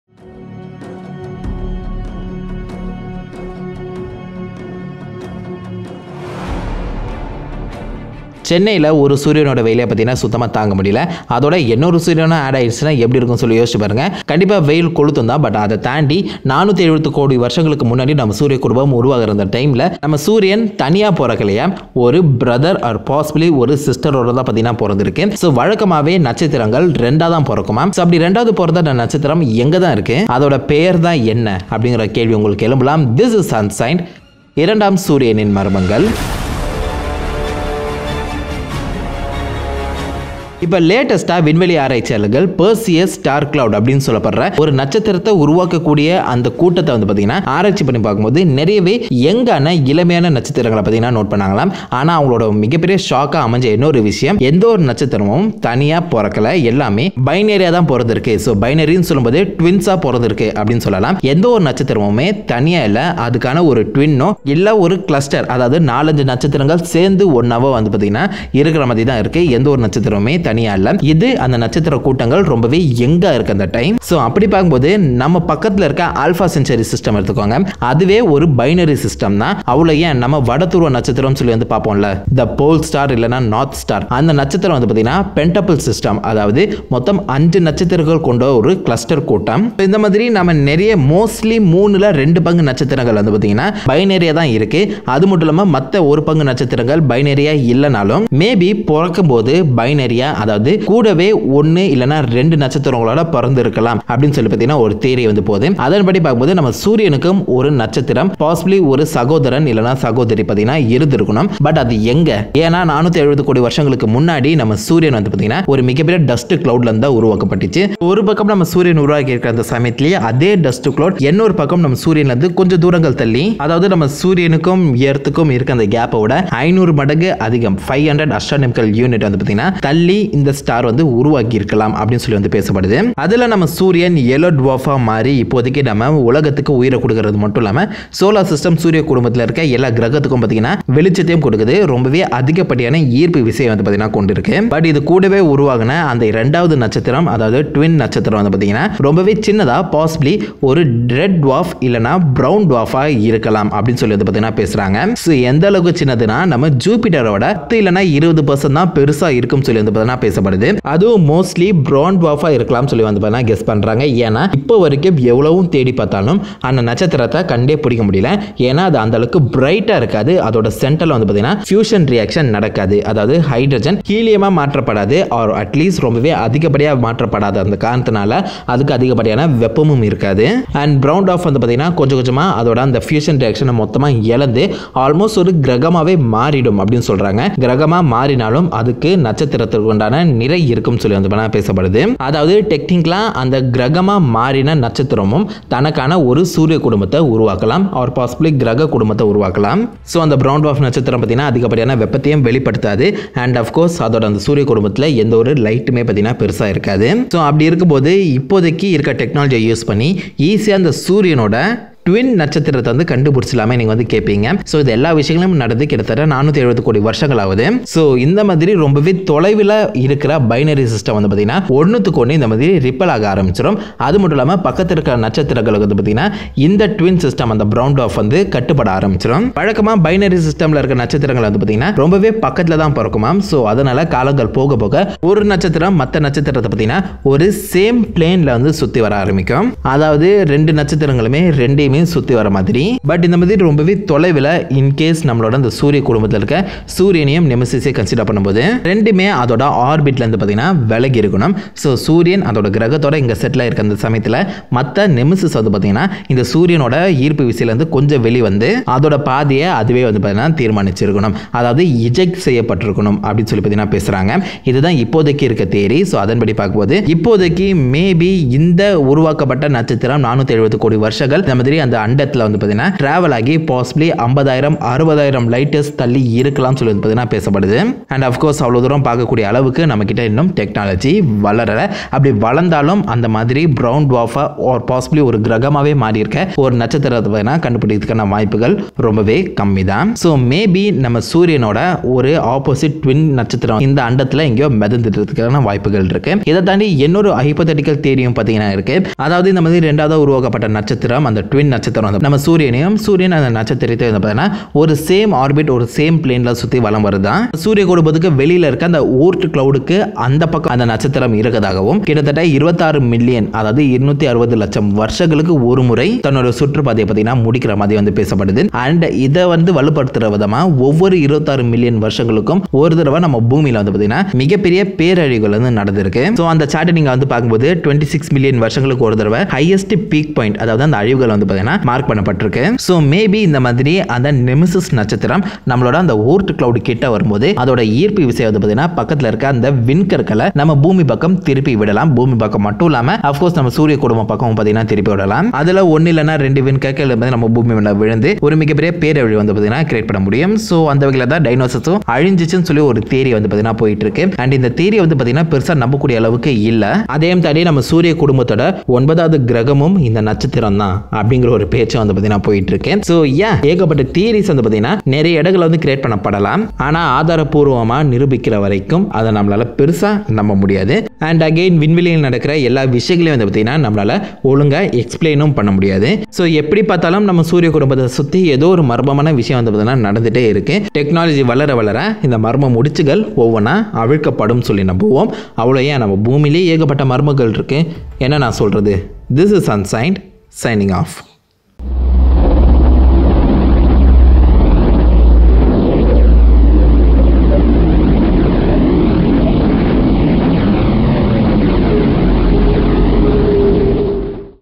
சூரியனுடன் சேர்ந்து பிறந்த இரண்டாம் நட்சத்திரம் - Nemesis Star
latest theory suggest that sun actually born with a sibling star nicknamed nemesis.